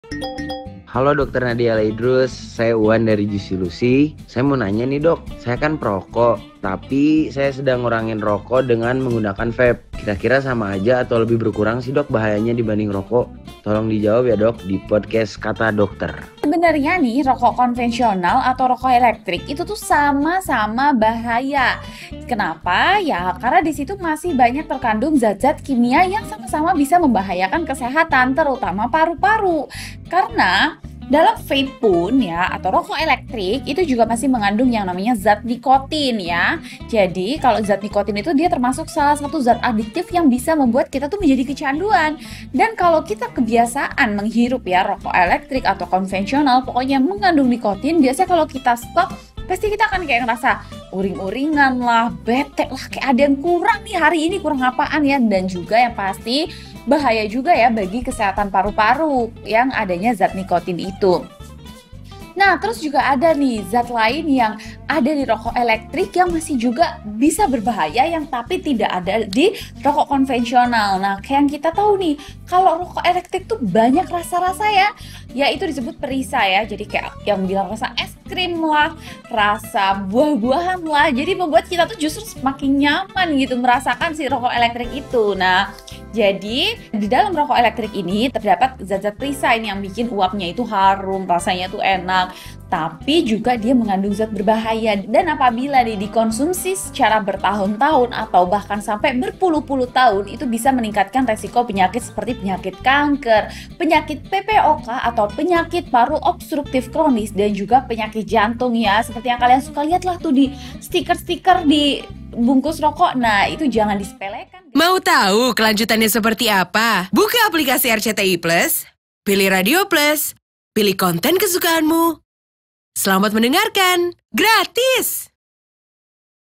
Bahaya Rokok Elektrik Yang Bisa Menyebabkan Penyakit Kanker dan Jantung
Katanya rokok elektrik lebih aman daripada rokok konvensional. Bener gak sih? Ternyata rokok elektrik dan rokok konvensional sama-sama bahaya bagi kesehatan, terutama kesehatan paru-paru dan jantung.

Dengerin Podcast Kata Dokter di RCTI+. Klik